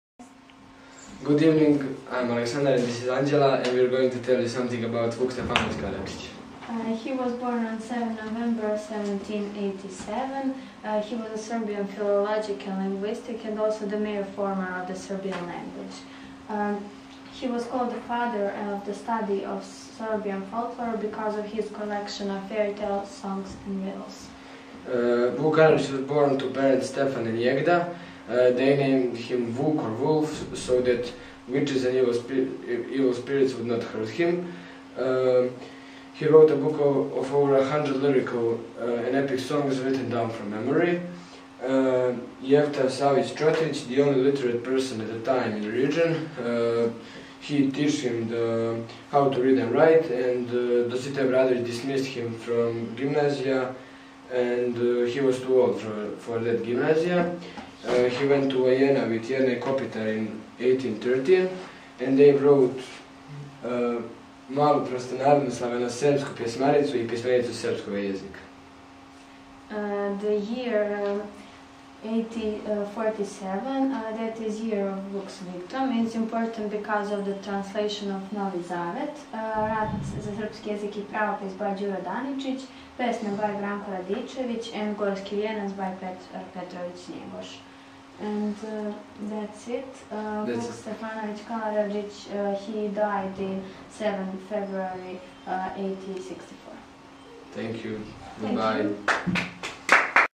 144 2016 LINGVA Serbian Historical Figures by Aleksandar Trifković and Anđela Mišković
Srpske istorijske ličnosti - Aleksandar Trifković and/i Anđela Mišković - Group/Grupa B2.b with teacher/sa nastavnicom Katarinom Andrić